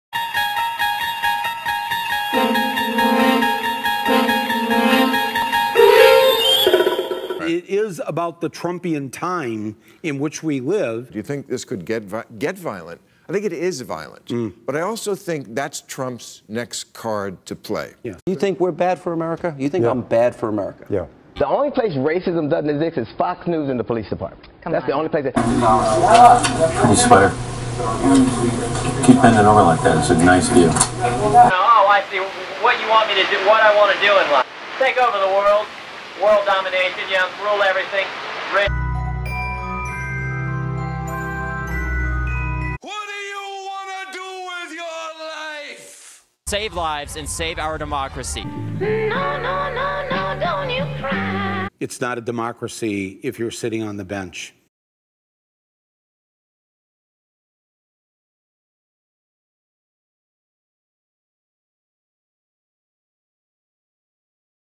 What You Want
Michael Moore couldn’t say it better: It’s not a democracy when you’re sitting on the bench. The recent few years, the Trumpian time, has been like The Twilight Zone. He continues to deceive, lie to the public, and lead the nation into the darkest, most divided time in recent history

Hate crimes have gone up since Trump took office. America has arguably never been more divided since the Civil War. Rhetoric of Dems and Republicans calling each other “bad for America” is rampant. Each side is obsessed with taking over.

Listen to the kids though. What do they want to do with their life? Save lives, and save our democracy. Maybe that’s what we really need right now